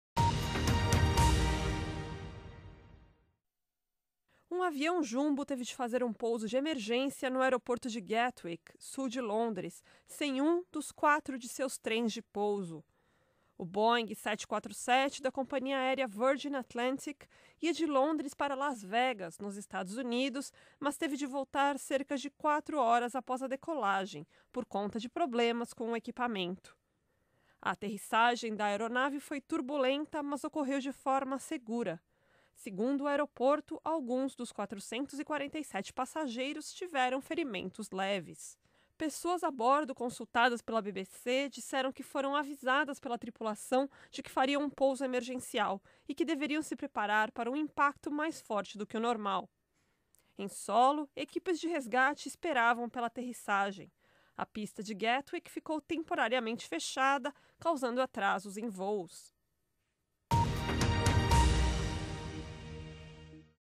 Avião faz aterrissagem de emergência em Londres sem trem de pouso
Jumbo da Virgin Atlantic voava de Gatwick a Las Vegas quando problema técnico obrigou aeronave a retornar; veja pouso.